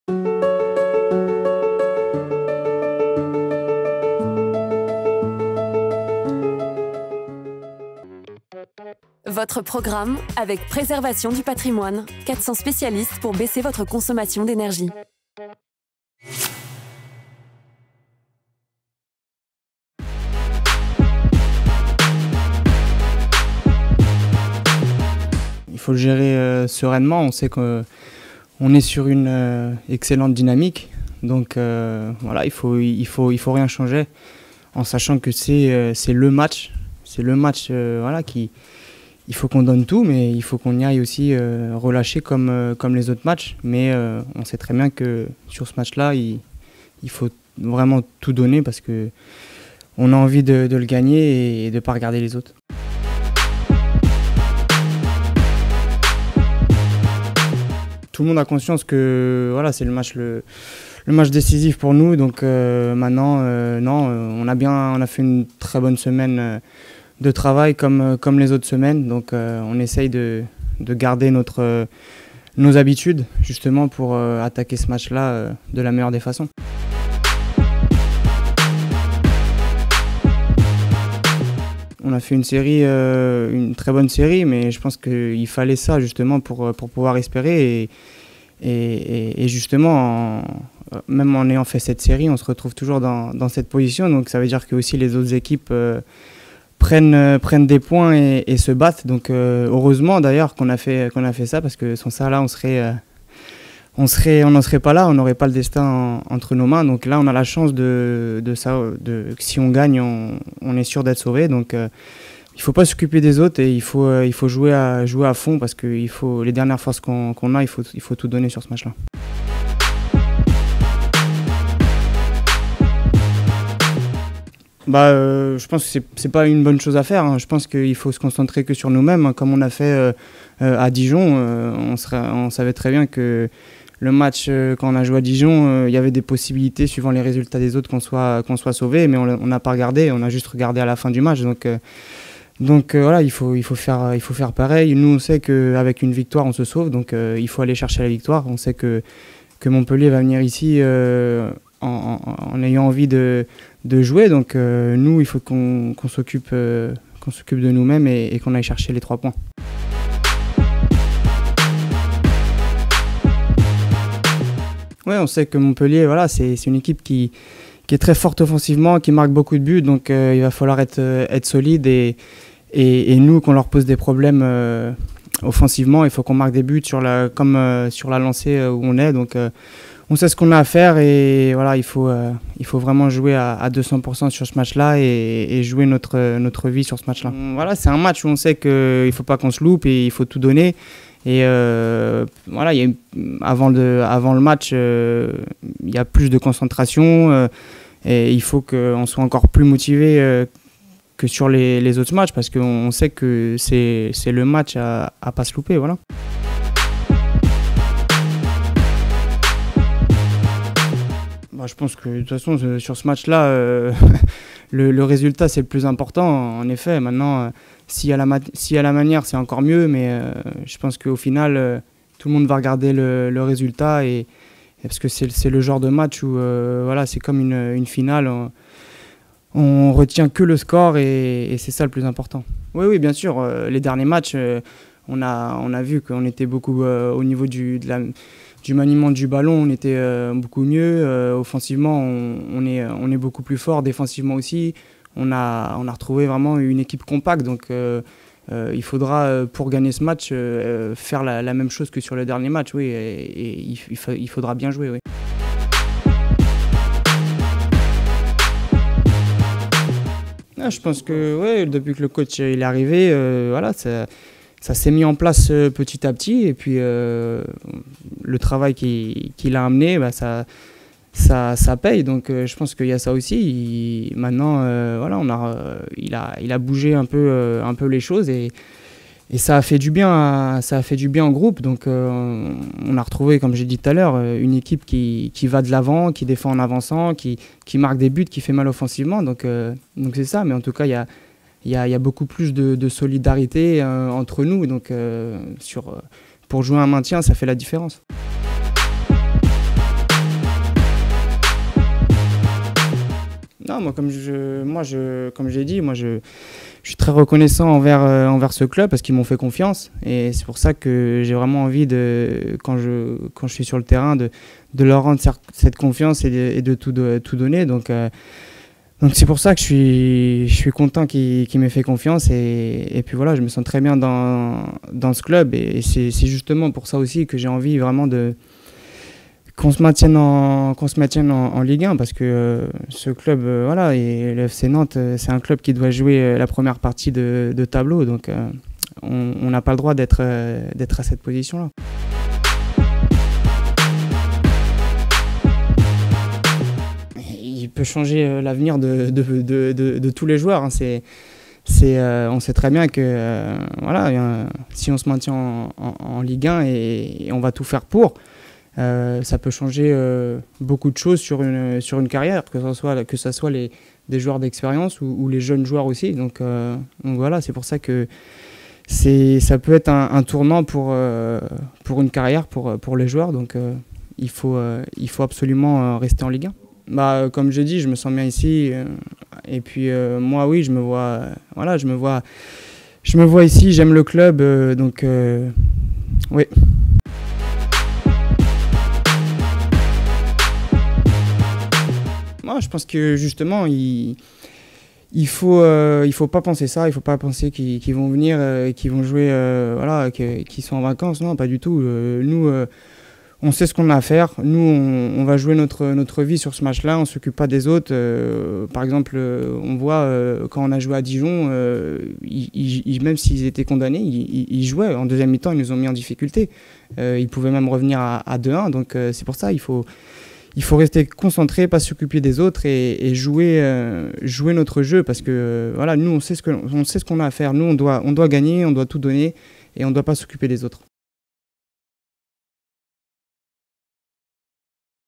Sébastien Corchia avant FC Nantes - Montpellier HSC
Présent en conférence de presse vendredi midi, Sébastien Corchia est revenu sur la rencontre à venir dimanche soir (21h), au Stade de La Beaujoire, pour le compte de la 38ème journée de Ligue 1 Uber Eats.